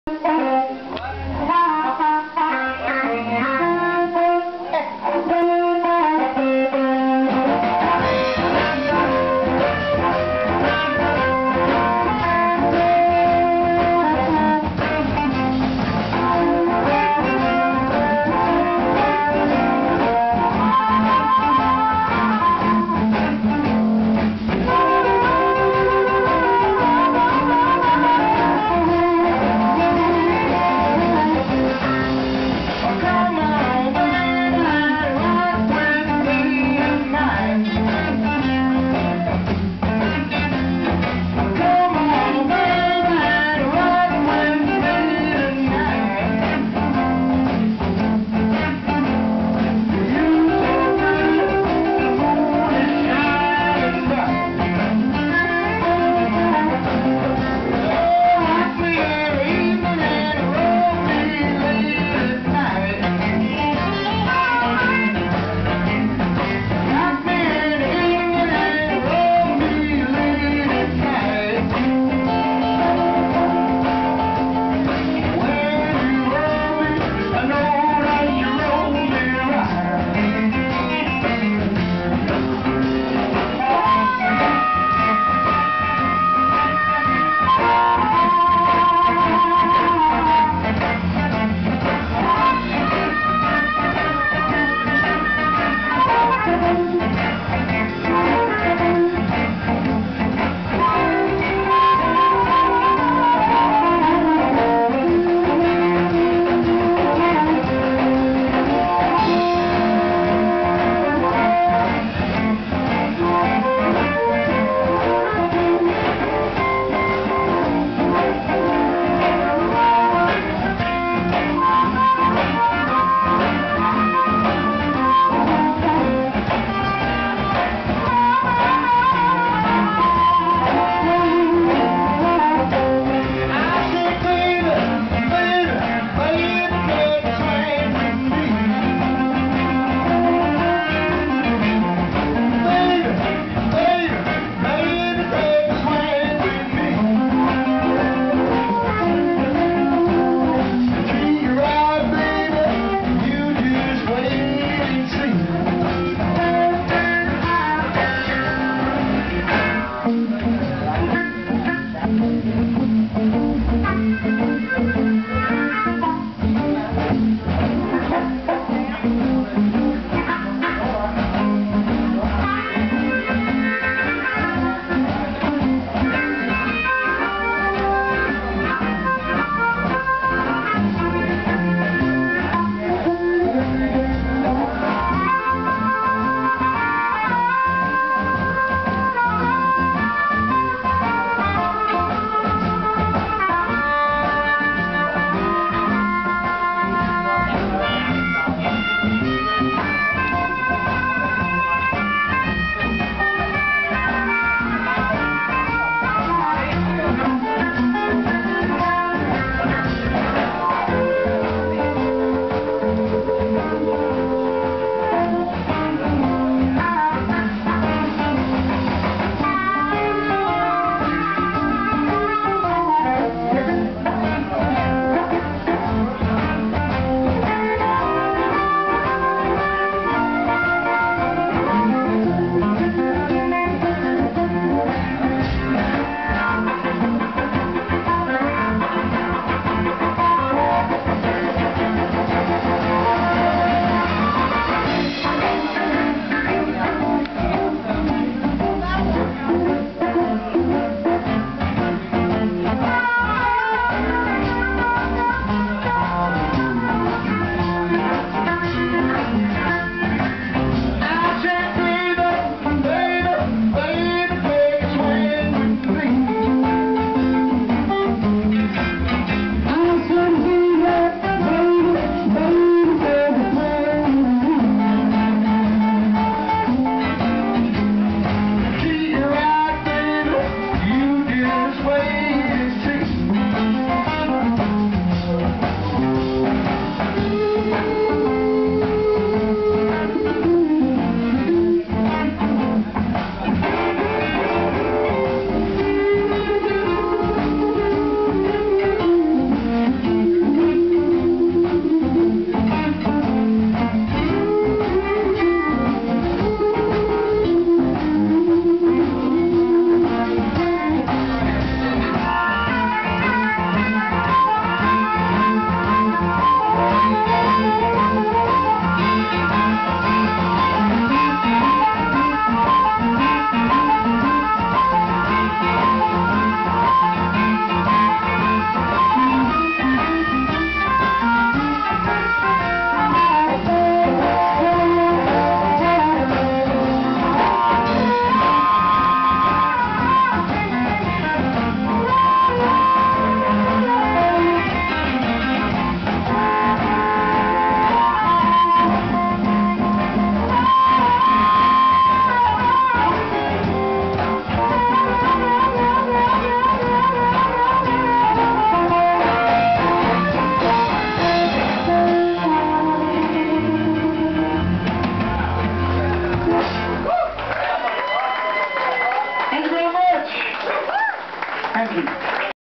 130924 Little Mike & The Tornadoes at Boston's on the Beach Blue Tuesday w/ Famous Frank #5
Boston's on the Beach: 40 S Ocean Blvd. (A1A), Delray Beach FL 33483 bostonsonthebeach.com (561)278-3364. Every Tue: Famous Frank Hosts Blue Tuesday w/ very, very special guests, Cover Charge $5-$10; Sept  24: Little Mike and The Tornadoes.
"Mike" Michael Markowitz - Vocals, Harp
Tony O Melio - Guitar, Harp, Vocals
Brad Vickers - Bass guitar
Danny Hamilton - Bass
Jonathan Buck - Drums
Sitting in tonight:
Frank Ward - Guitar, Vocals
Daniel Lombardi  - Guitar
Jazz & Blues Florida - Florida's online guide to live jazz & blues in clubs, concerts, and